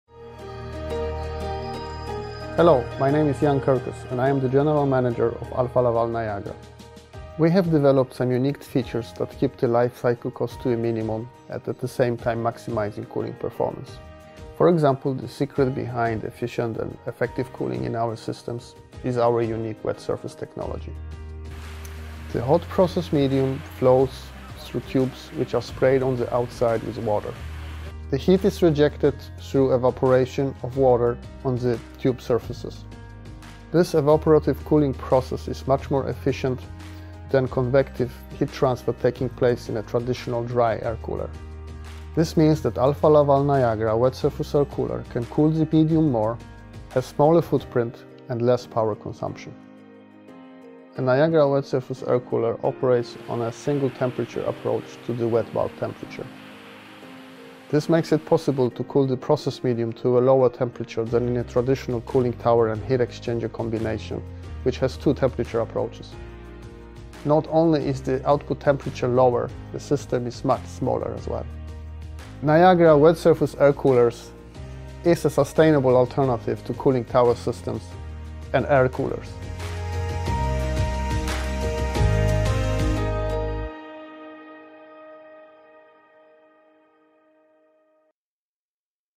Effective and efficient cooling with Alfa Laval Niagara wet surface air coolers
The secret behind the effective and efficient cooling in the Alfa Laval Niagara wet surface air cooler systems is our unique WetSurface technology. Thanks to the unique features that we have developed you can keep life cycle cost to a minimum while maximizing cooling performance.

The evaporative cooling process used in Niagara wet surface air coolers is much more effective than the convective heat transfer taking place in a traditional dry air cooler. This means that we can help you cool the process medium more, and the system is significantly smaller and has lower power consumption.